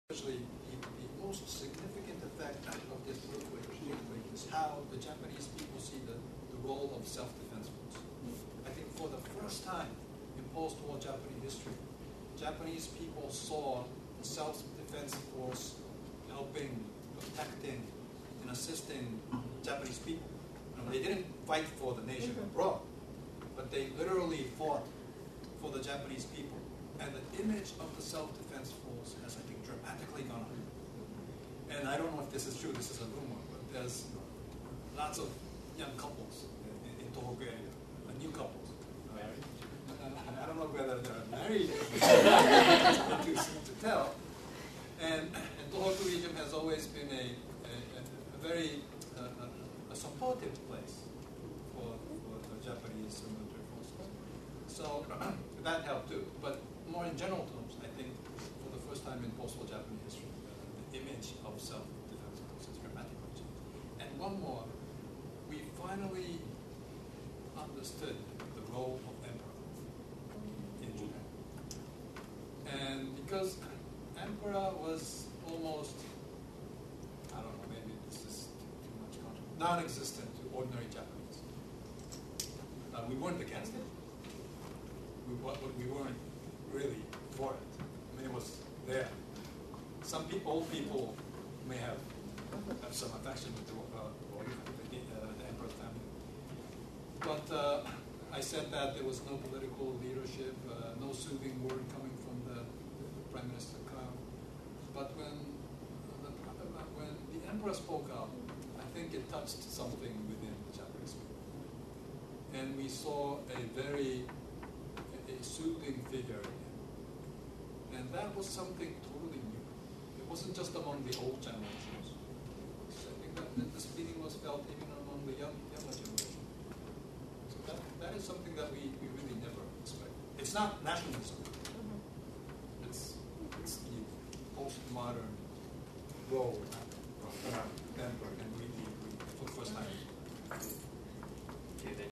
Japan After the Earthquake- The SDF and the Emperor
Professor Toshihiro Nakayama discusses the changes in the Japanese
peoples' perception of the Self-Defense Force and the Emperor after the
earthquake.